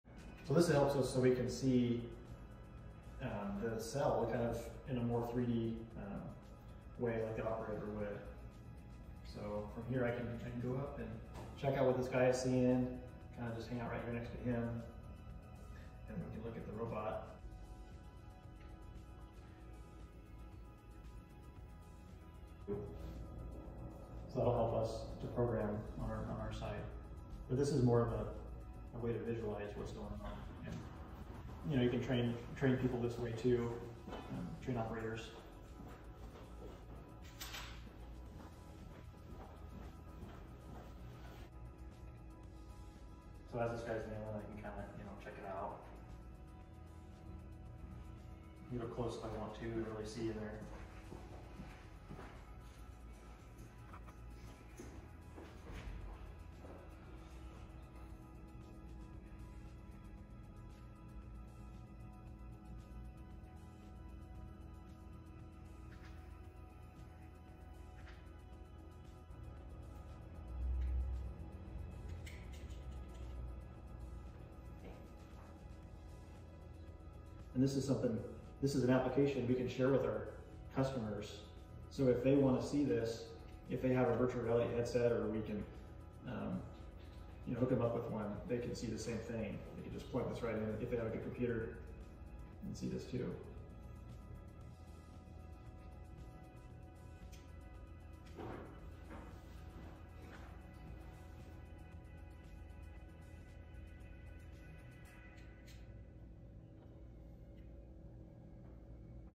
Using VR to Operate Robot Simulation
Check out one of our mechanical engineers using a virtual reality headset and controls to operate an ABB RobotStudio simulation we designed!
Zach describes some of the capabilities we have with this advanced technology and how it benefits our customers.